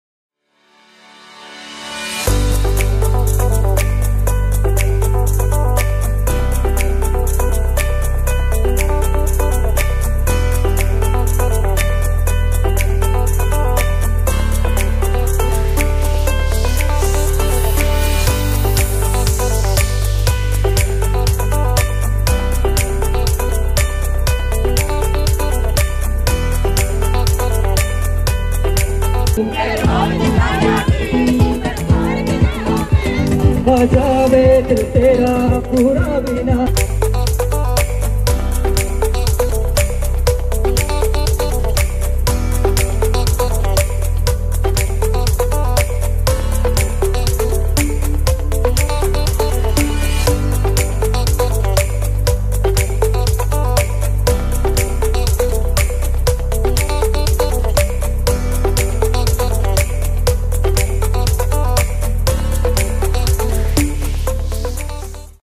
Emaar Employees Family Night: Reliving Spectacular Moments at Oceanfront Beach!
Unveiling the highlights from our spectacular Emaar Employees Family Night! Join the nostalgia as we revisit the laughter, games, and unforgettable moments that lit up the night at the Emaar Oceanfront Beach. Relive the magic with us!